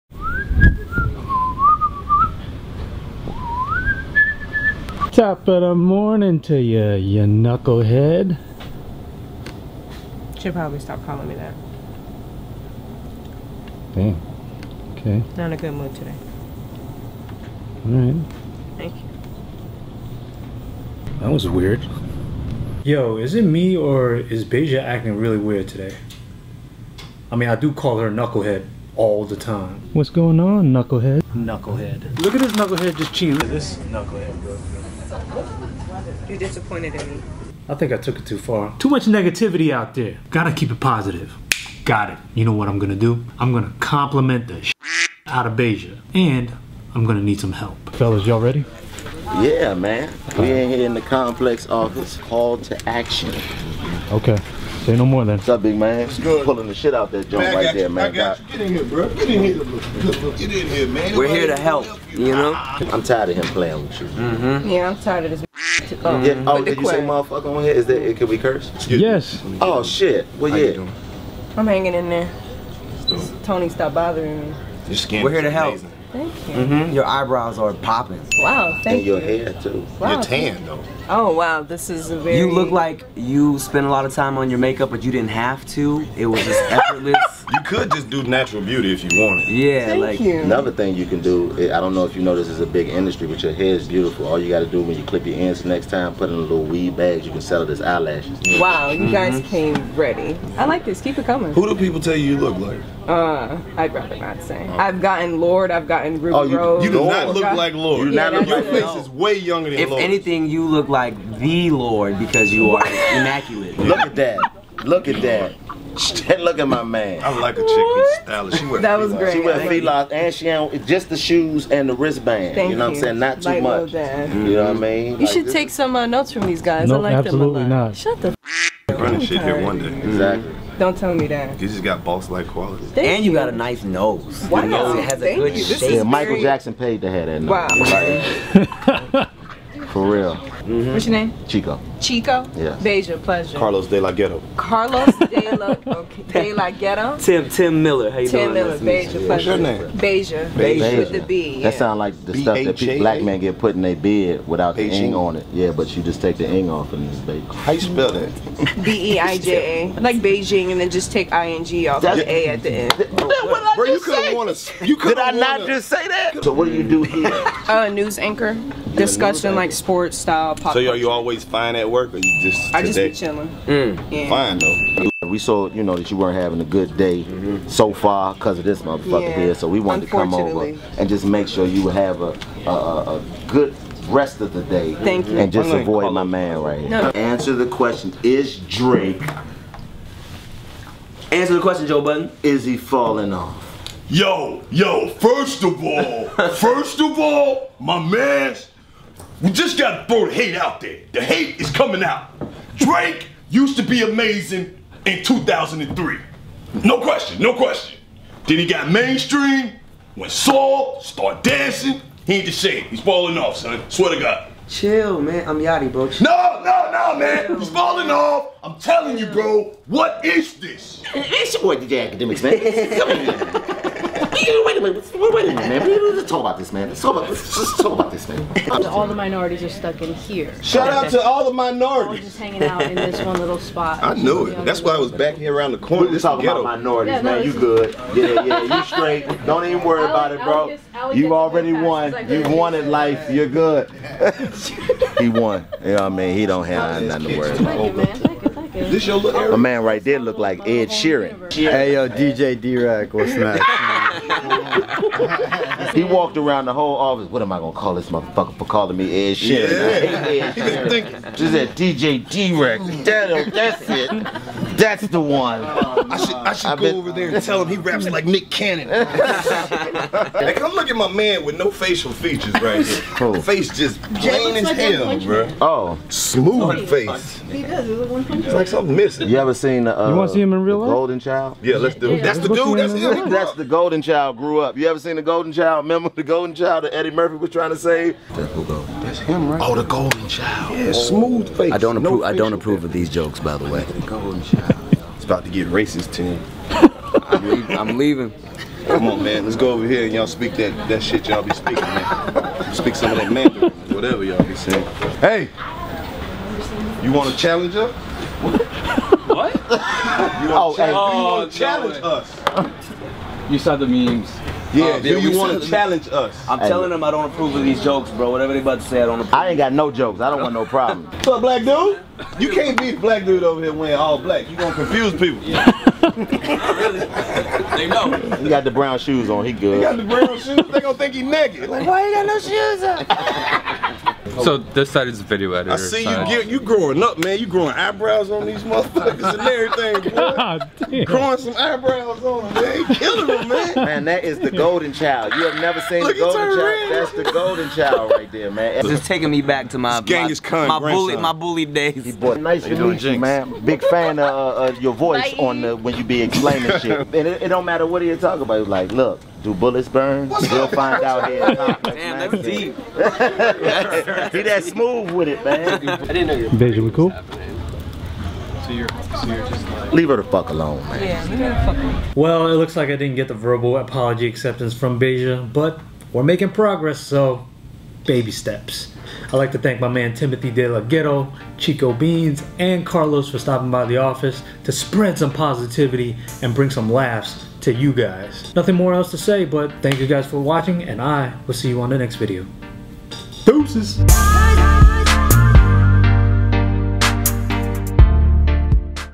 COMPLIMENTING THE SH*T OUTTA BEIJA VELEZ WITH THE WILD 'N OUT CAST!
On today’s episode, Tony gets on Beija’s bad side and decides to make up for it by doing something nice with a little help from Timothy DeLaGhetto, Chico Bean, and Karlous Miller of ‘Wild 'N Out.’ Later, the ‘Wild 'N Out’ crew do their best impression of ‘Everyday Struggle’ and get in an office roast session. Be sure to catch the season premiere of ‘Wild 'N Out’ tonight on MTV at 11 p.m. ET.